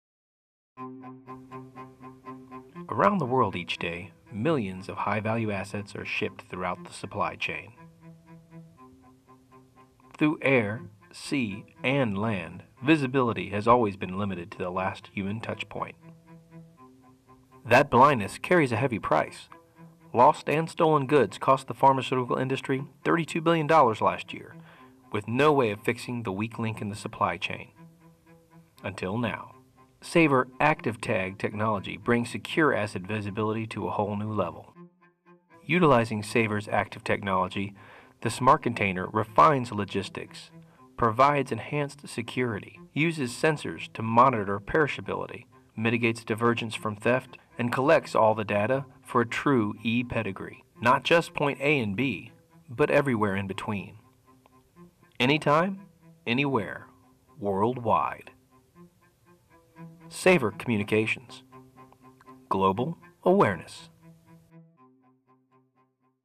Global Awareness by SAVR Communications
Demonstration Video: Going beyond traditional RFID. Utilizing the latest RF technology to provide global track and trace capability for pharmaceuticals and other high value assets. SAVR technology provides in-transit visibility throughout the supply chain.